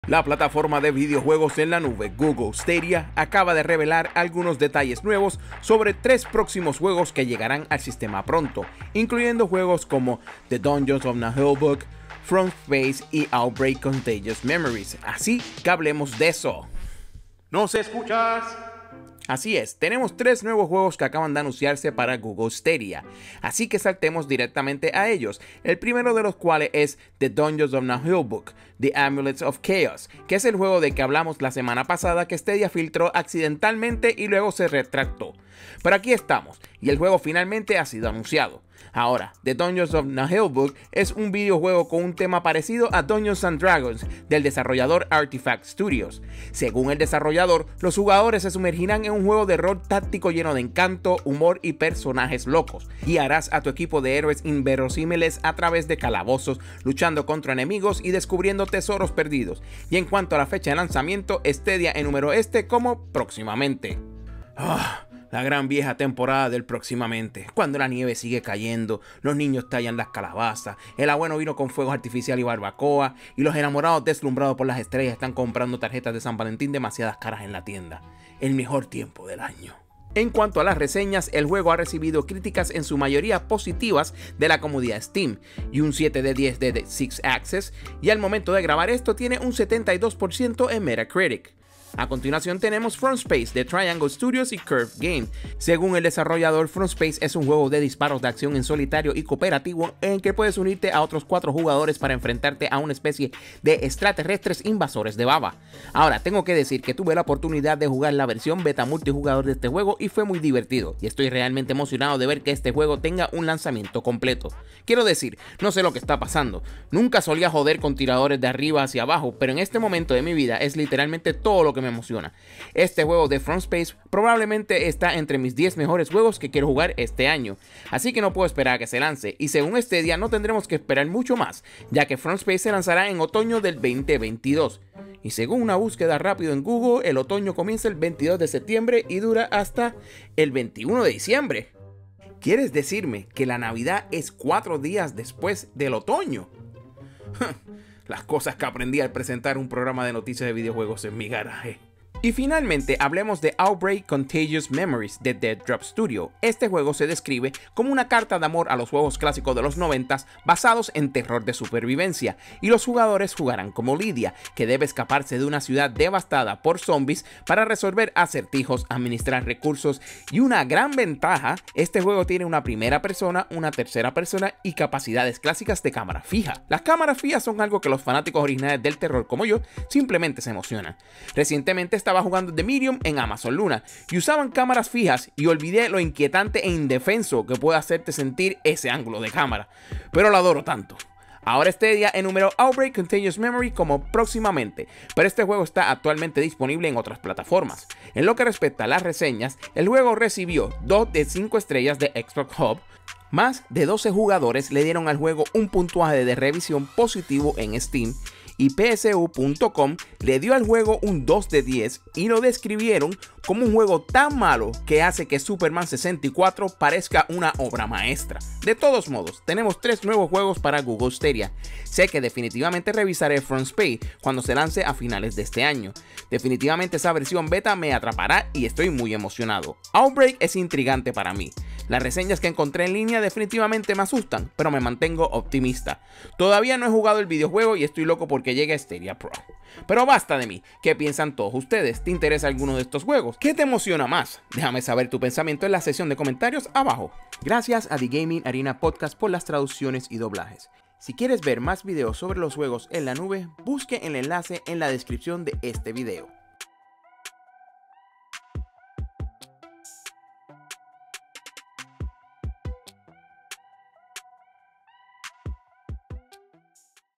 ¡Extraterrestres, calabozos y zombis! Stadia anuncia 3 nuevos juegos - The Nerf Report en Español
La plataforma de videojuegos en la nube, Google Stadia, acaba de revelar algunos detalles nuevos sobre tres próximos juegos que llegarán al sistema pronto. Incluyendo juegos como The Dungeon Of Naheulbeuk, From Space y Outbreak: Contagious Memories.